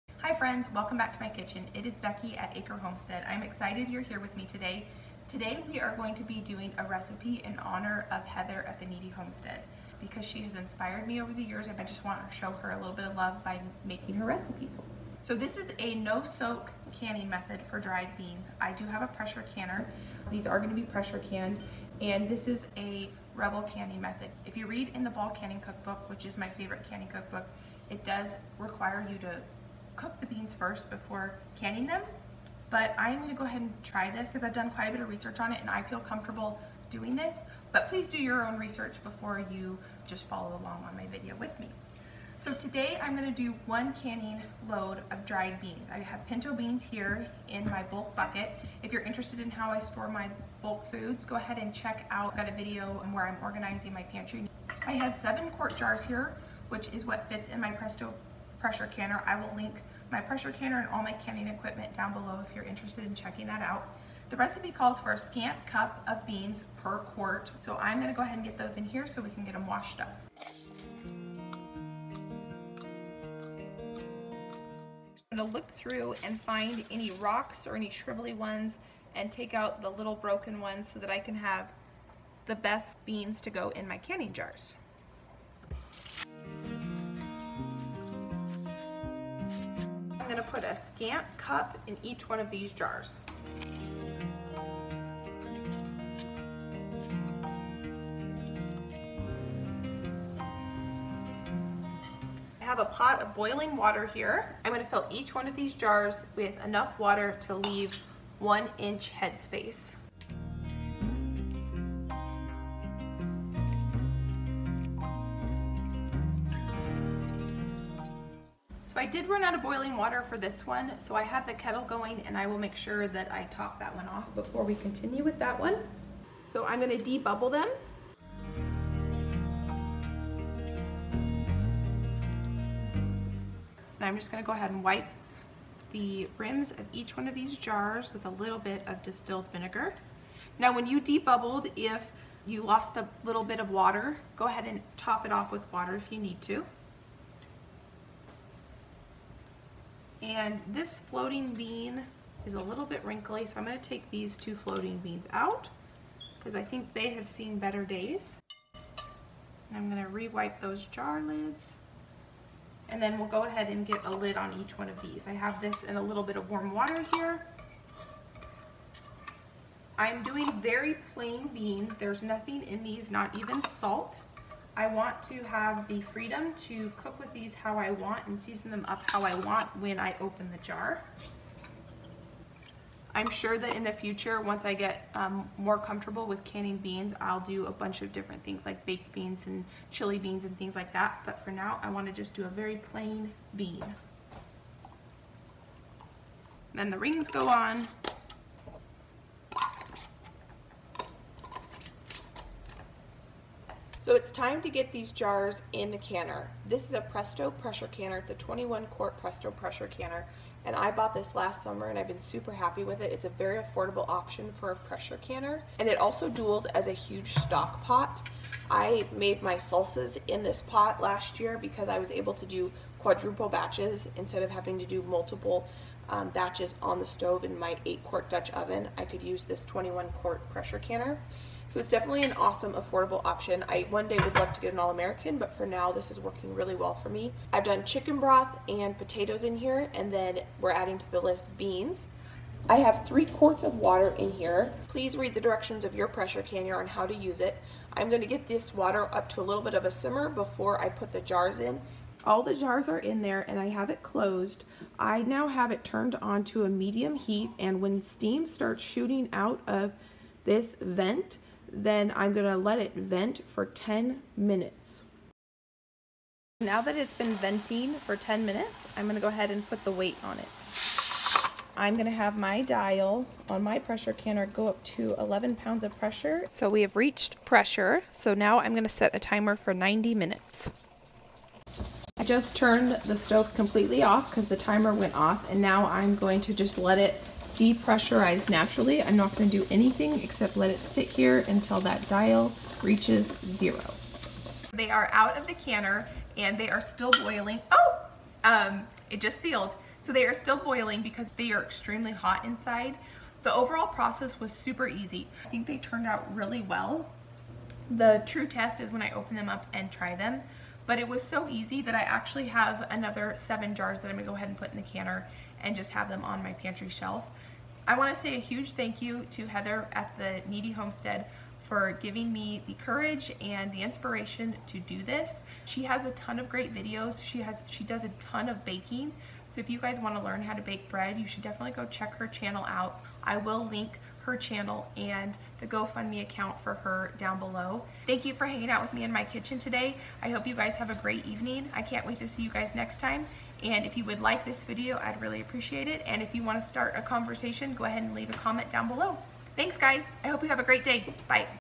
Pressure Canning Dried Pinto Beans | No Soak Method | Rebel Canning l Inspired by Kneady Homestead
This video is about canning dried pinto beans with Heather's, from the Kneady Homestead, method.

23 Quart Presto Pressure Canner
Ball Canning Cook Book
Canning Supplies

Follow me for real time updates on Instagram @AcreHomestead